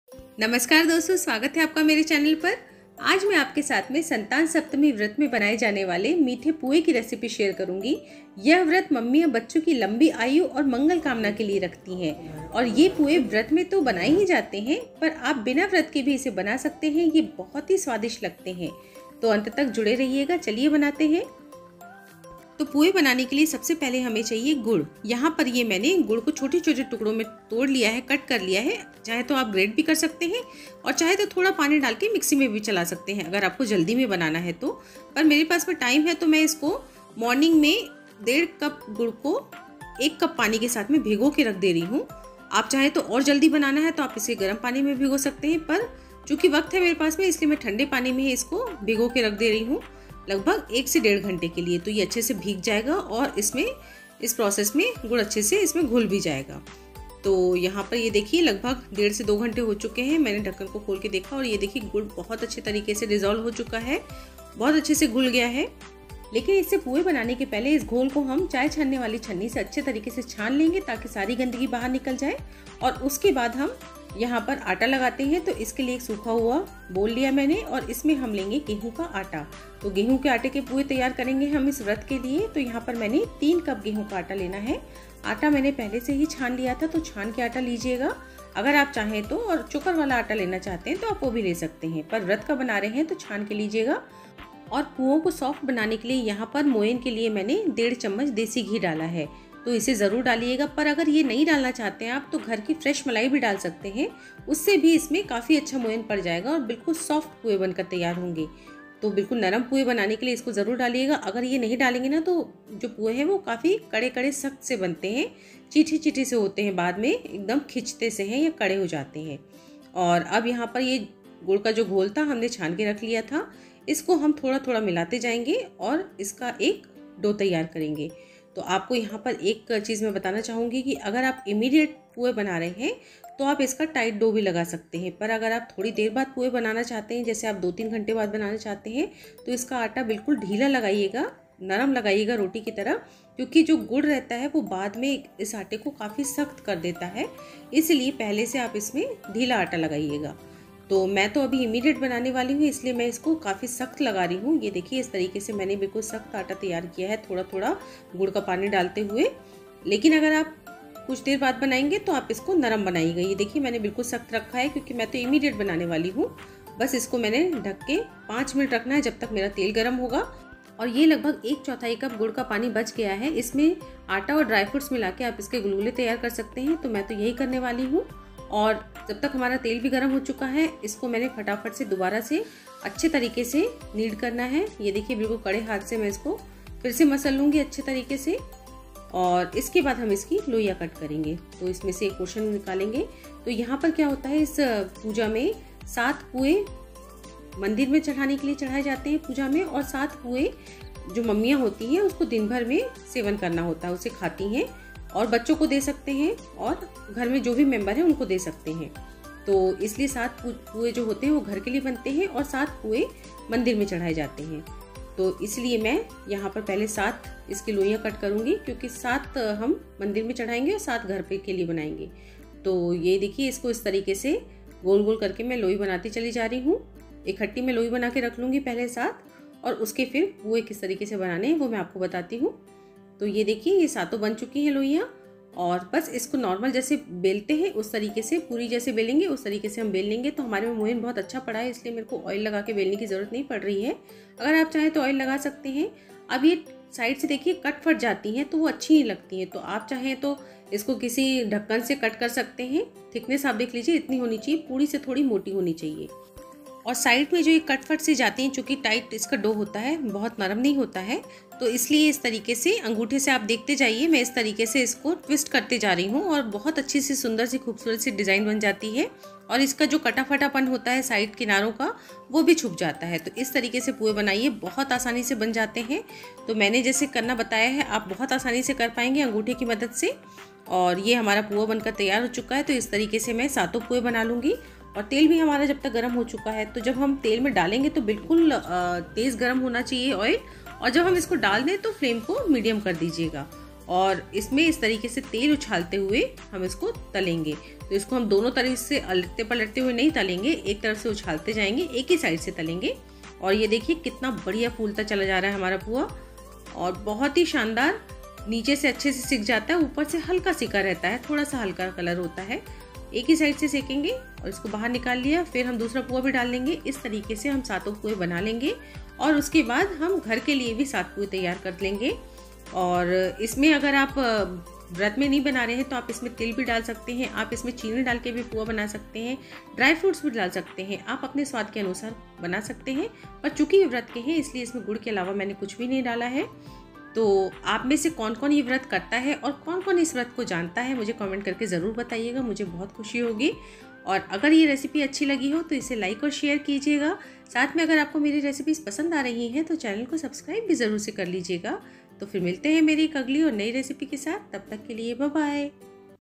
Santan Saptami Special - Meethe Pua Recipe | संतान सप्तमी व्रत - गुड़ और आटे के मीठे पुआ | Gulgule
Meethi Puri Recipe | Santan Saptami Special Meethi Puri

Hello Everyone,
आज मैं आपके साथ में संतान सप्तमी व्रत में बनाए जाने वाले गुड और आटे के मीठे पुए की रेसिपी शेयर करूंगी। यह मीठी पूरी आप व्रत में बना सकते हैं और बिना व्रत के भी तिल और शक्कर को डालकर भी बना सकते हैं। Santan Saptami Vrat बच्चों की मंगल कामना और दीर्घायु के लिए किया जाता है।

Taste Of States